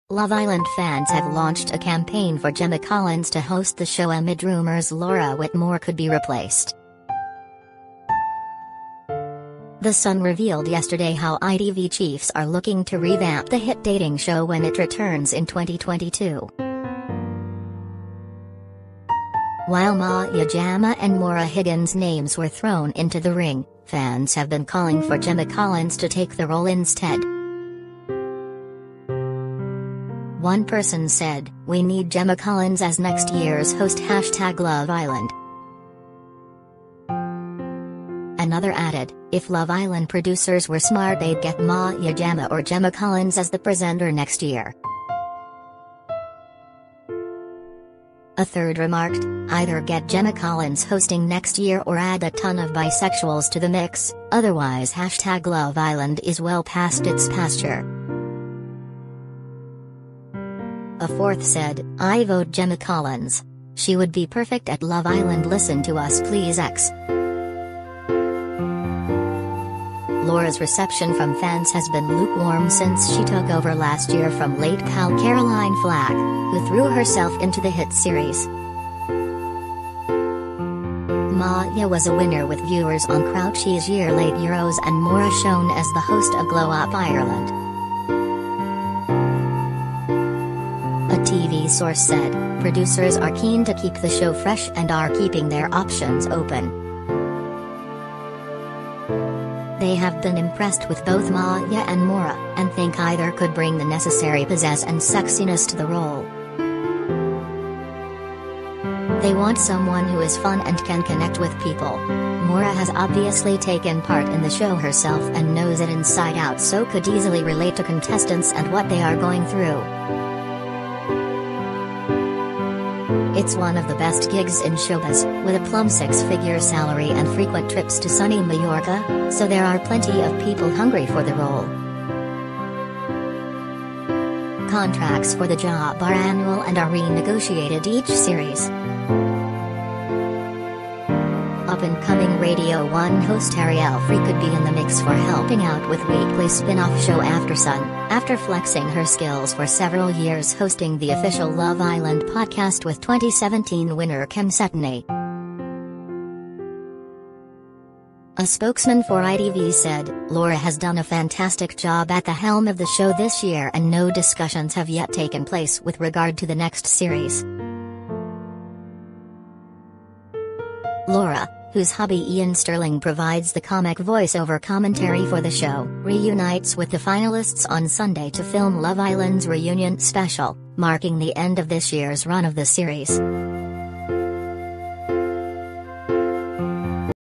HIDDEN GEM Love Island fans launch campaign for Gemma Collins to host amid talk Laura Whitmore could
LOVE Island fans have launched a campaign for Gemma Collins to host the show amid rumours Laura Whitmore could be replaced.
The Sun revealed yesterday how ITV chiefs are looking to revamp the hit dating show when it returns in 2022.
While Maya Jama and Maura Higgins' names were thrown into the ring, fans have been calling for Gemma Collins to take the role instead.
One person said: "We NEED Gemma Collins as next year’s host LoveIsland"
Another added: "If love island producers were smart they’d get maya jama or Gemma Collins as the presenter next year"
A third remarked: "Either get Gemma Collins hosting next year or add a ton of bisexuals to the mix, otherwise LoveIsland is well past its pasture!"
A fourth said: "i vote gemma collins! she would be perfect @LoveIsland listen to us please x"
Laura’s reception from fans has been lukewarm since she took over last year from late pal Caroline Flack, who threw herself into the hit series.
Maya was a winner with viewers on Cro